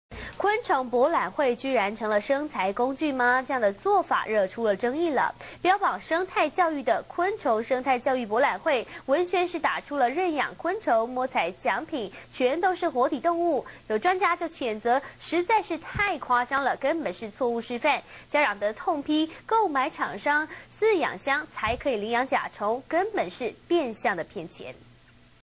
買箱才能領養甲蟲 昆蟲博覽會挨批騙錢
中視新聞》昆蟲博覽會竟成生財工具？標榜生態、教育的「昆蟲生態教育博覽會暨螢火蟲特展」，文宣打出「認養昆蟲」，摸彩獎品全是活體動物，專家譴責「太扯！根本是錯誤示範。」家長則痛批購買廠商飼養箱才能領養甲蟲，是「變相騙錢」。